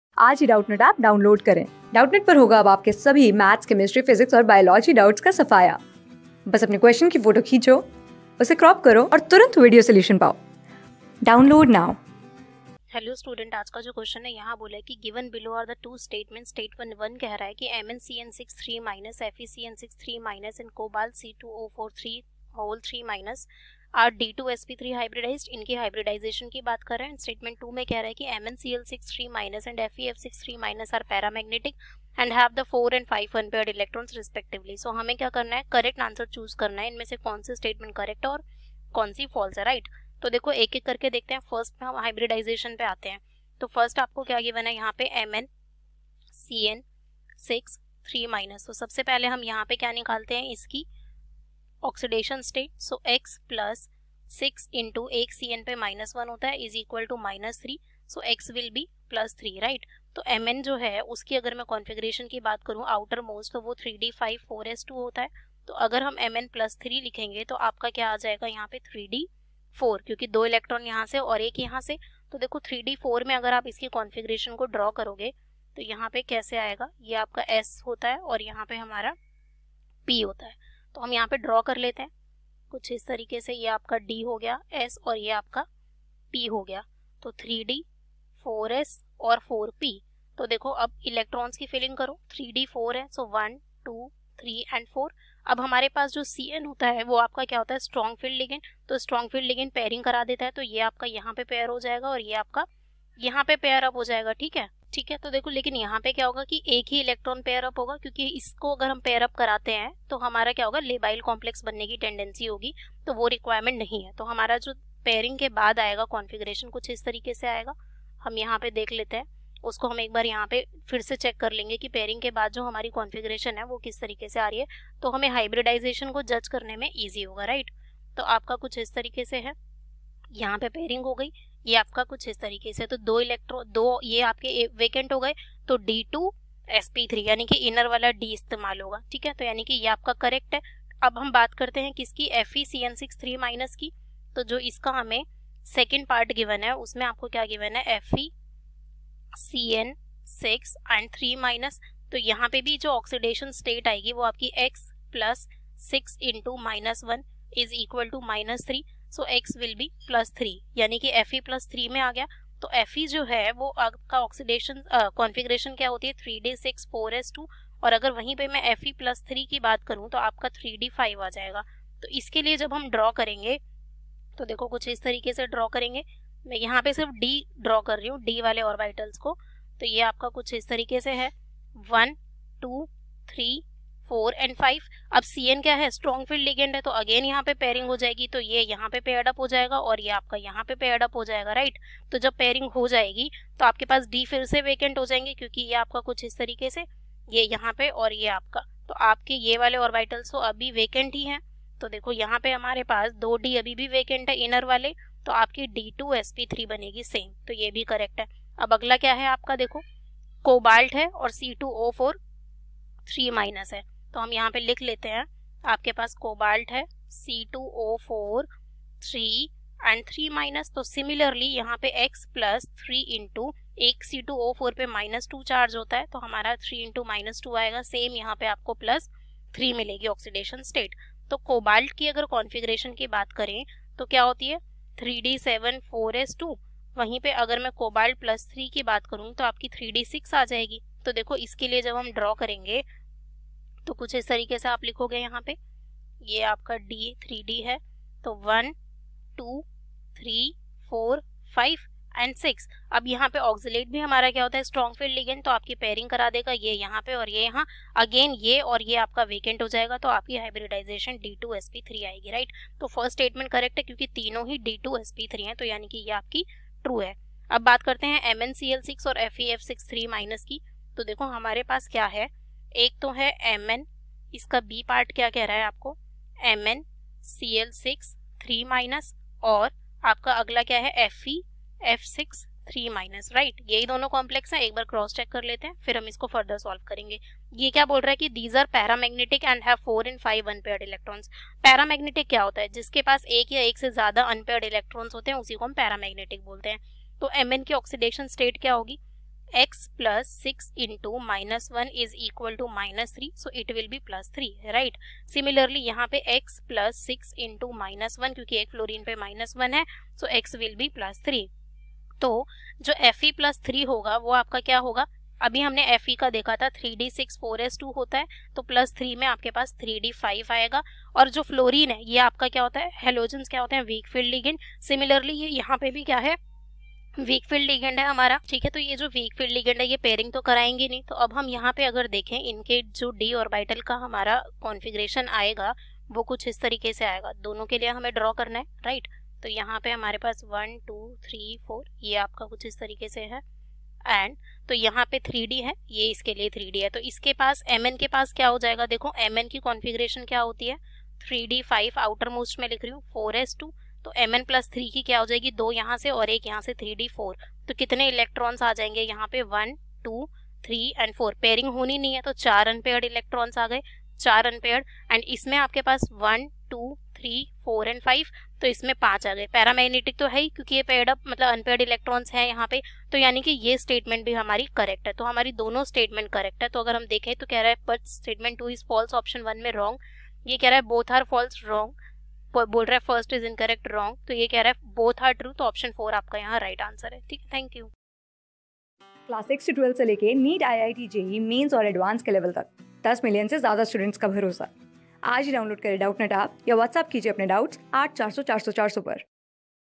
Given below are two statements : Statement I: [Mn ( CN ) _(6) ] ^(3-) , [Fe (CN) _(6) ] ^(3-) an...
Given below are two statements : Statement I: [Mn ( CN ) _(6) ] ^(3-) , [Fe (CN) _(6) ] ^(3-) and [Co (C _(2) O_(4)) _(3) ] ^(3-)  are d ^(2) sp ^(3) hybridised. Statement II: [Mn Cl _(6) ] ^(3-) and [Fe F_(6)]^(3-) are paramagnetic and have 4 and 5 unpaired electrons,respectively. In the light of the above statements, choose thecorrect answer from the options given below :
Class: 12
Subject: CHEMISTRY
Chapter: JEE MAINS 2021
Board:IIT JEE

👉 Have Any Query? Ask Us.